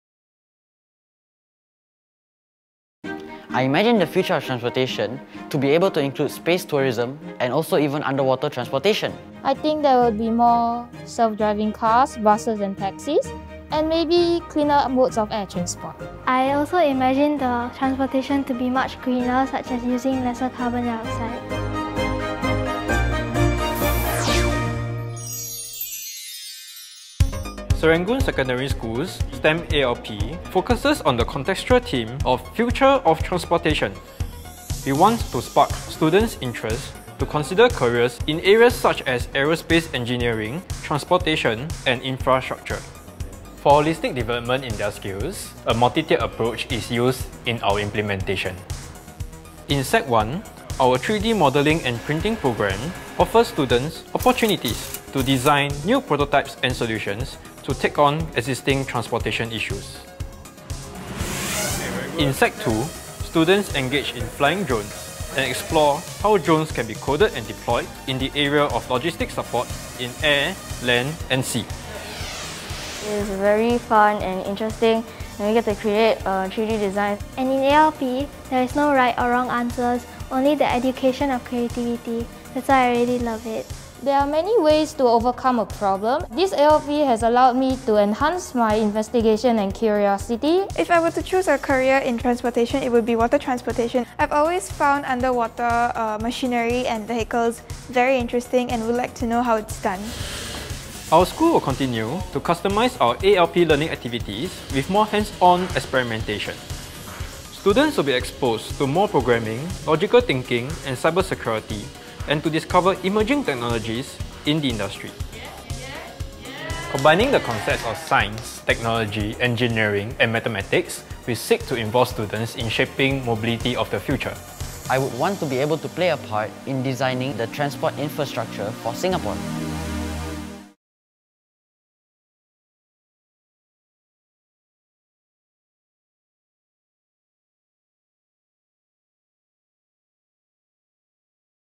Happenings@Serangoon Secondary School
Students from Serangoon Secondary School imagine what the future of transportation looks like.